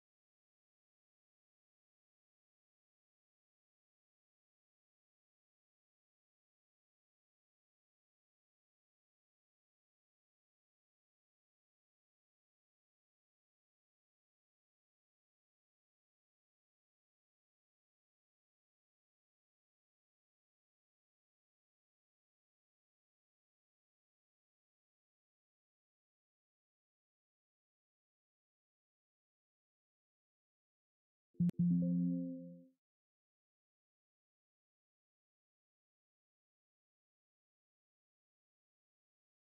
How to download and save your Pic-time gallery
How to download and save your images sent to you with pictime.